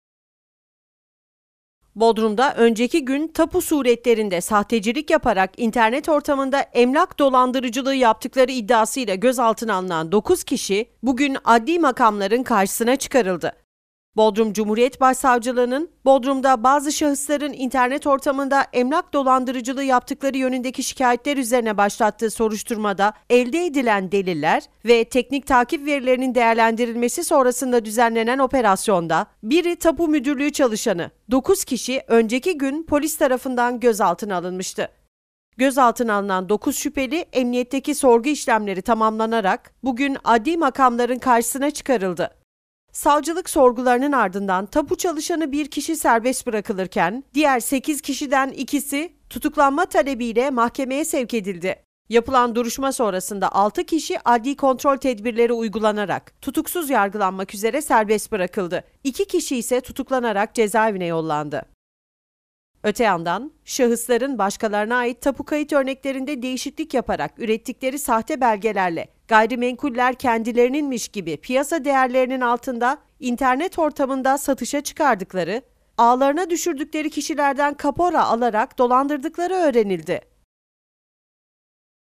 DOLANDIRICILIK OPERASYONUNDA 2 TUTUKLU!
Dolandırıcılık Operasyonunda 2 Tutuklu!
Bodrum'da önceki gün tapu suretlerinde sahtecilik yaparak internet ortamında emlak dolandırıcılığı yaptıkları iddiasıyla gözaltına alınan 9 kişi, bugün adli makamların karşısına çıkarıldı.
Bodrum Cumhuriyet Başsavcılığı'nın, Bodrum'da bazı şahısların internet ortamında emlak dolandırıcılığı yaptıkları yönündeki şikayetler üzerine başlattığı soruşturmada elde edilen deliller ve teknik takip verilerinin değerlendirilmesi sonrasında düzenlenen operasyonda, 1'i tapu müdürlüğü çalışanı 9 kişi önceki gün polis tarafından gözaltına alınmıştı.

Gözaltına alınan 9 şüpheli, emniyetteki sorgu işlemleri tamamlanarak bugün adli makamların karşısına çıkarıldı.

Savcılık sorgularının ardından tapu çalışanı bir kişi serbest bırakılırken, diğer 8'den 2 kişi tutuklanma talebiyle mahkemeye sevk edildi. Yapılan duruşma sonrasında 6 kişi adli kontrol tedbirleri uygulanarak tutuksuz yargılanmak üzere serbest bırakıldı, 2 kişi ise tutuklanarak cezaevine yollandı.

Öte yandan, şahısların başkalarına ait tapu kayıt örneklerinde değişiklik yaparak ürettikleri sahte belgelerle, gayrimenkuller kendilerininmiş gibi piyasa değerlerinin altında internet ortamında satışa çıkardıkları, ağlarına düşürdükleri kişilerden kapora alarak dolandırdıkları öğrenildi.